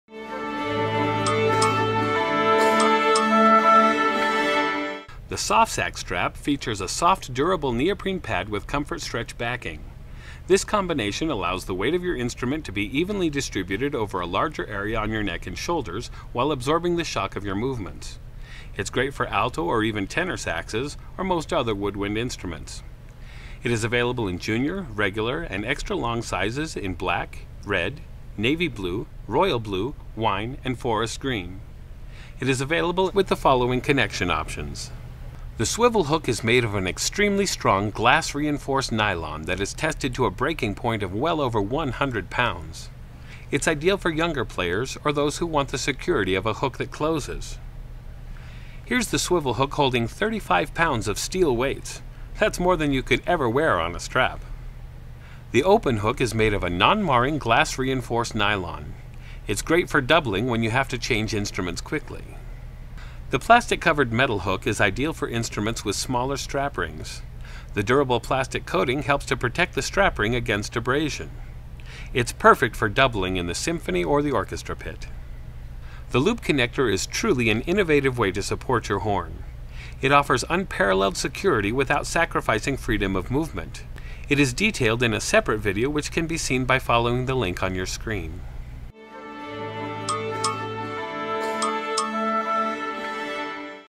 Soft Sax Strap® - Product Peek - Neotech
The revolutionary Soft Sax® Strap actually helps reduce uncomfortable neck and shoulder pressure, fatigue and bounce. The innovative design incorporates neoprene with a comfort-stretch backing. This combination allows the weight of your instrument to be evenly distributed over a larger area on your neck and shoulders while absorbing the shock of your movements. This is how the Soft Sax® Strap makes your instrument feel 50% lighter and 100% more comfortable. You'll definitely feel the difference!